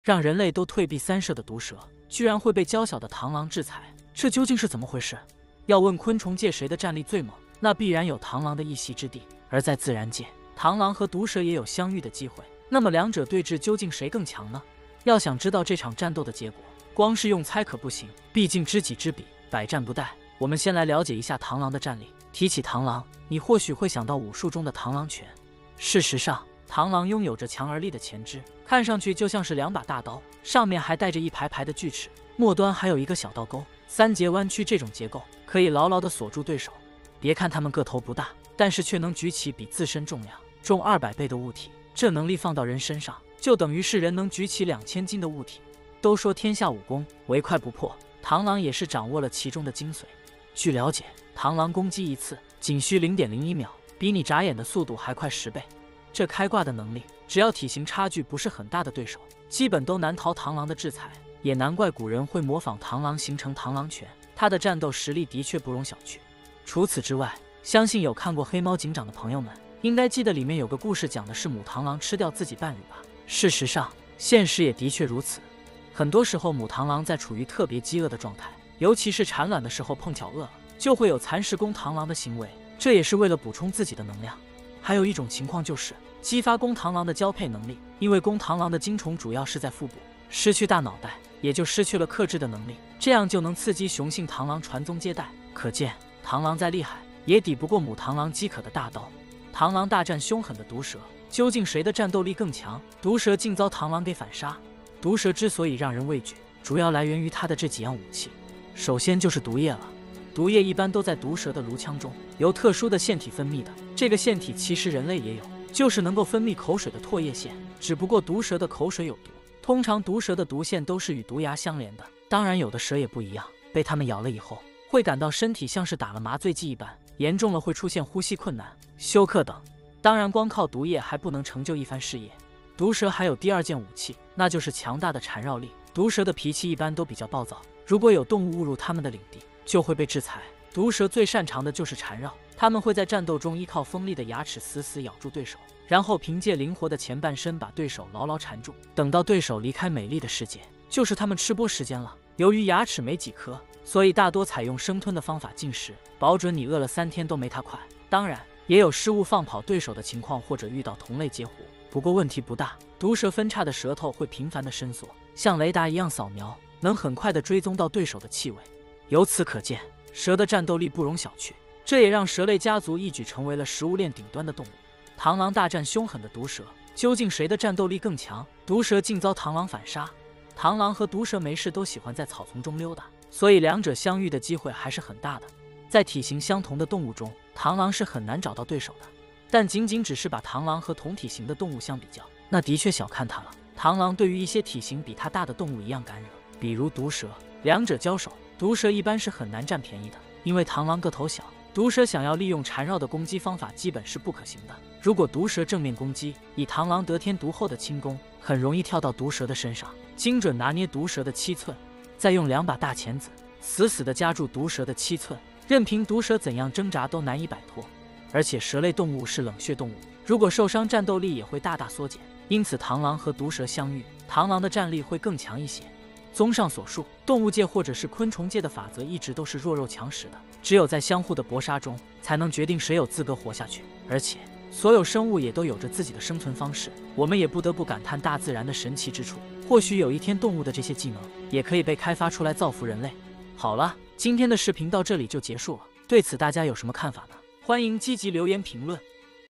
螳螂大戰凶狠的毒蛇，究竟誰的戰鬥力更強？毒蛇竟遭螳螂反殺？ 【趣普聞新】
🎉歡迎大家來到【趣普聞新】
有趣的科普🗿
新穎的感悟🎊
建立嶄新的🎃脈絡體系
我會給大家帶來不一樣的科普解說視頻📢